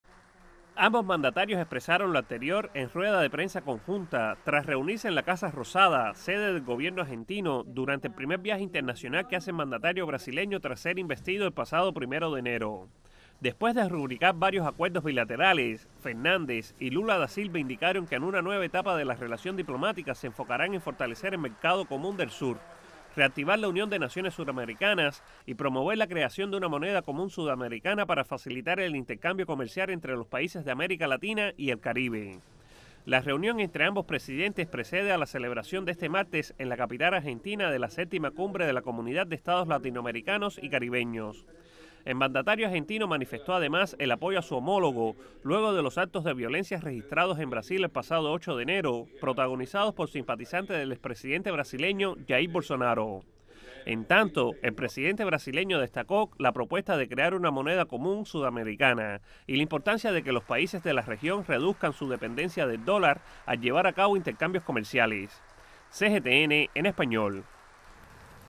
Argentina y Brasil se comprometen a relanzar vínculos bilaterales y promover integración regional
Los presidentes de Argentina, Alberto Fernández, y de Brasil, Luiz Inácio Lula da Silva, se comprometieron el 23 de enero a relanzar de forma más profunda las relaciones bilaterales y trabajar en conjunto para promover la integración regional, difundió la Presidencia argentina.

Ambos mandatarios expresaron lo anterior en rueda de prensa conjunta, tras reunirse en la Casa Rosada, sede del Gobierno argentino, durante el primer viaje internacional que hace el mandatario brasileño tras ser investido el pasado 1º de enero. Después de rubricar varios acuerdos bilaterales, Fernández y Lula da Silva indicaron que en una nueva etapa de la relación diplomática se enfocarán en fortalecer el Mercado Común del Sur, reactivar la Unión de Naciones Suramericanas y promover la creación de una moneda común sudamericana para facilitar el intercambio comercial entre los países de América Latina y el Caribe. La reunión entre ambos presidentes precede a la celebración del 24 de enero en la capital argentina de la séptima Cumbre de la Comunidad de Estados Latinoamericanos y Caribeños. El mandatario argentino manifestó además el apoyo a su homólogo, luego de los actos de violencia registrados en Brasil el pasado 8 de enero, protagonizados por simpatizantes del expresidente brasileño, Jair Bolsonaro. En tanto, el presidente brasileño destacó la propuesta de crear una moneda común sudamericana y la importancia de que los países de la región reduzcan su dependencia del dólar, al llevar a cabo intercambios comerciales.